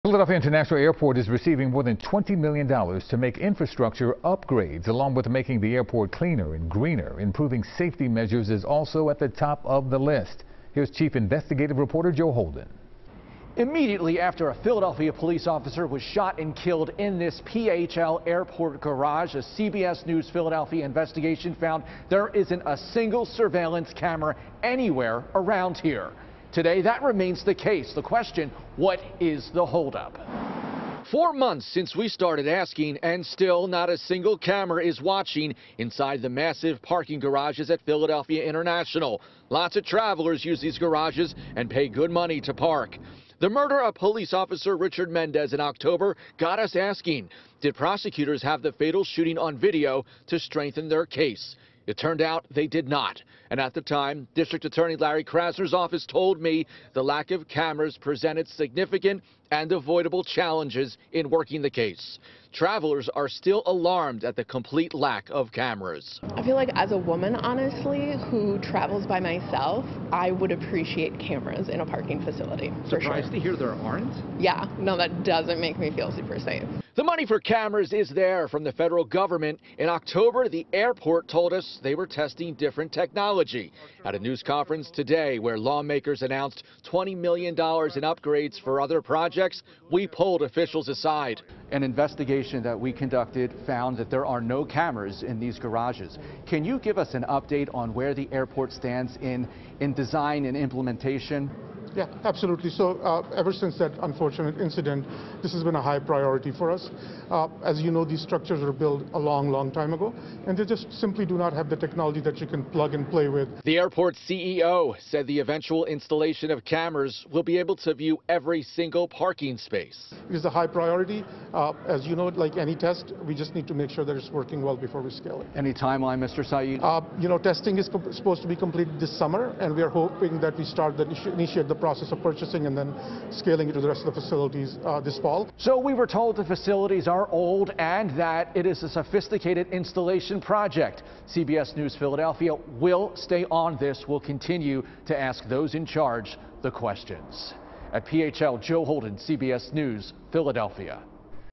What is the holdup of surveillance cameras being installed at PHL?
Philadelphia International Airport is receiving more than $20 million to make infrastructure upgrades. Along with making the airport cleaner and greener, improving safety measures is also at the top of the list. Joe Holden reports.